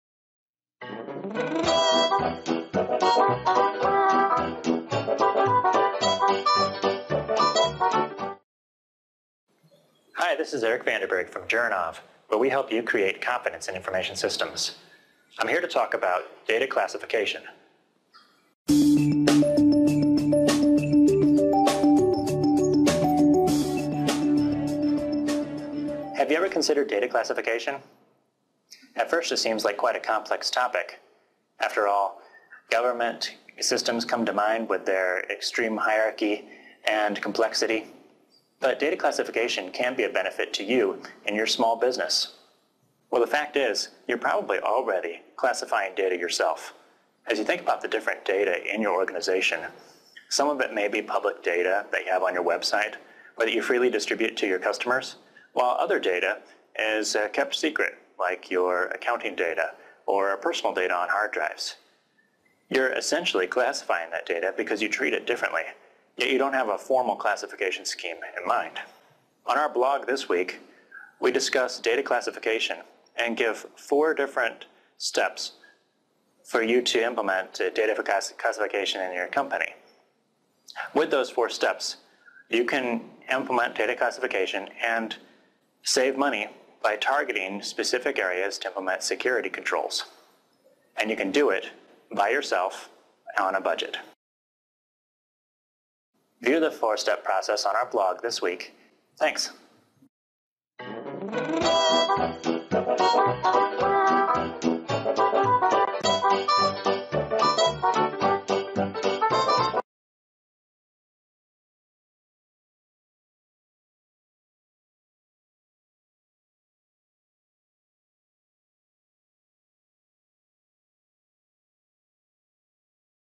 Achieving the Benefits of Data Classification on a Budget
Eric Vanderburg of JurInnov talks about data classification and information security in his newest blog post